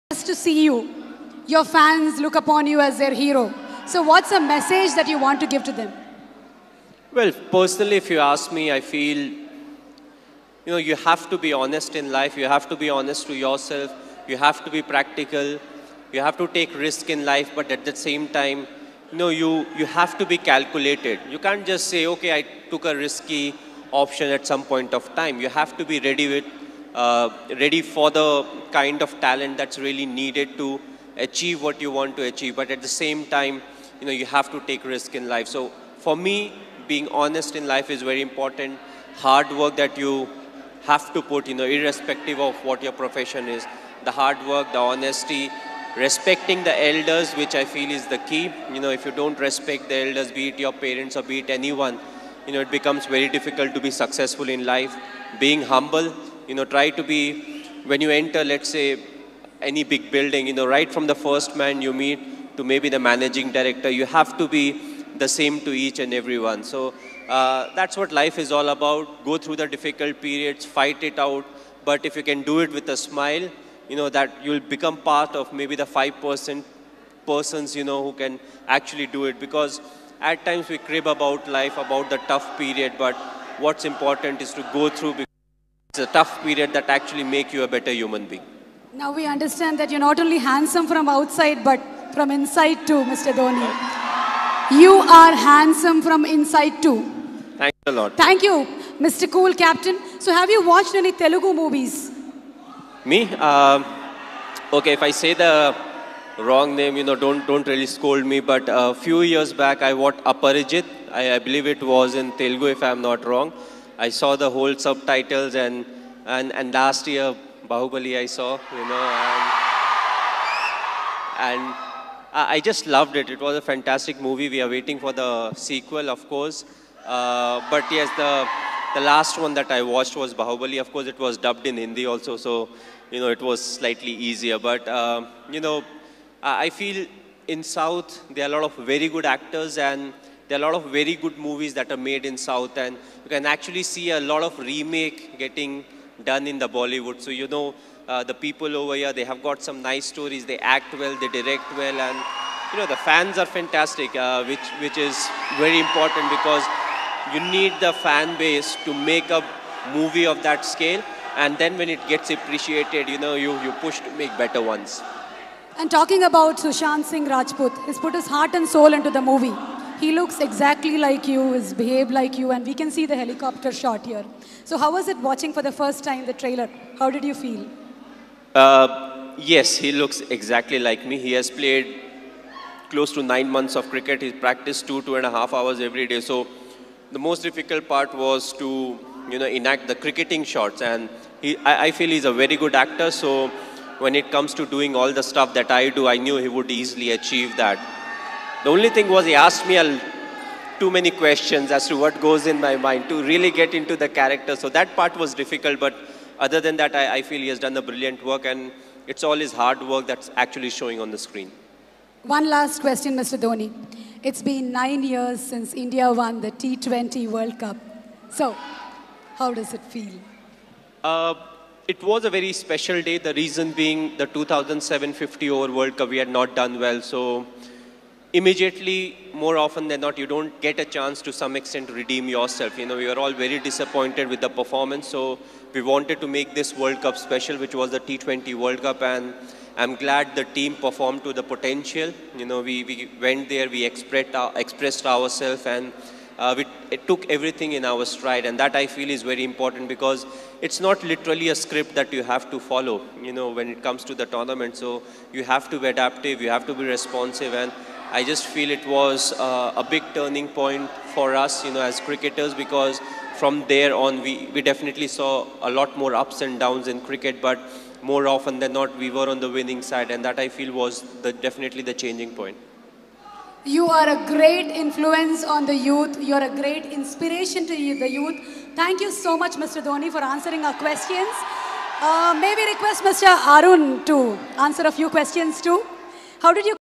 MS DHONI - MOTIVATION
This video is about ms dhoni talks about hardwork and consistency
Hope everyone will  enjoy it